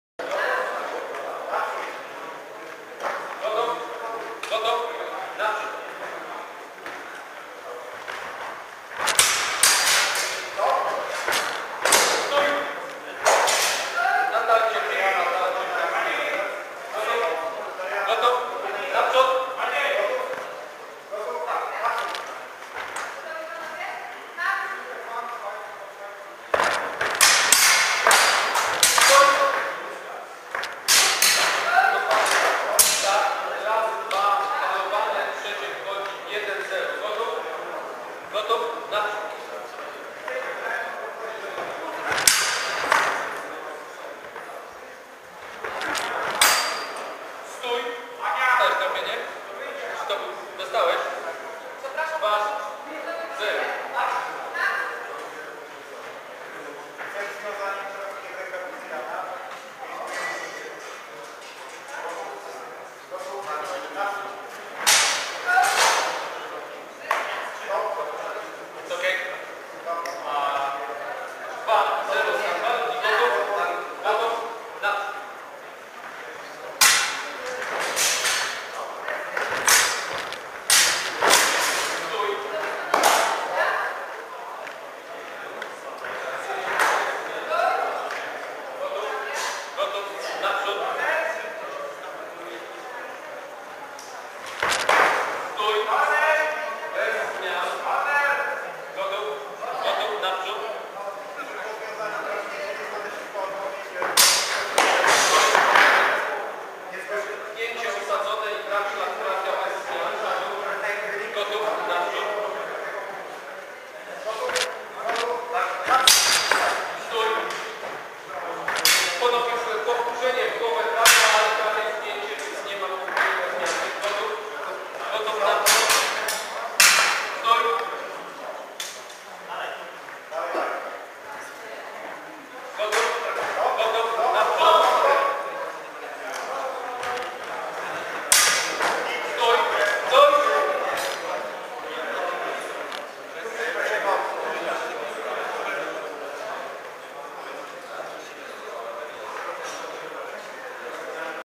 Jan Cgodkiewicz vs Maciej Zając - długi miecz ŚKUNKS 2012
Walka z jednej z czterech grup eliminacyjnych turnieju długiego miecza.
19 maja Rybnik.